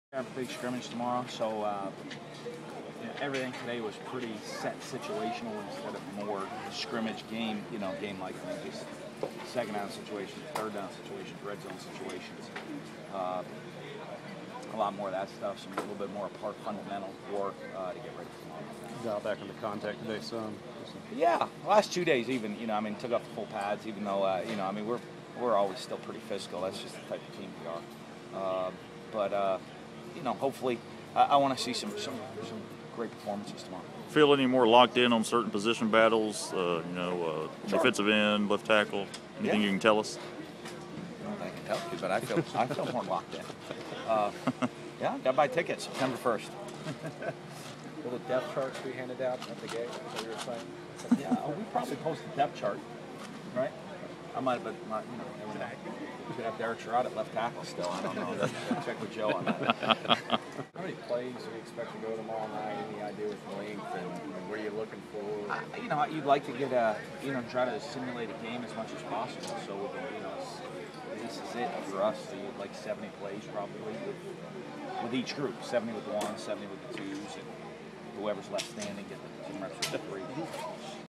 Dan Mullen 8.18.11
Mississippi State coach Dan Mullen speaks with reporters following practice, Thursday, Aug. 18, 2011.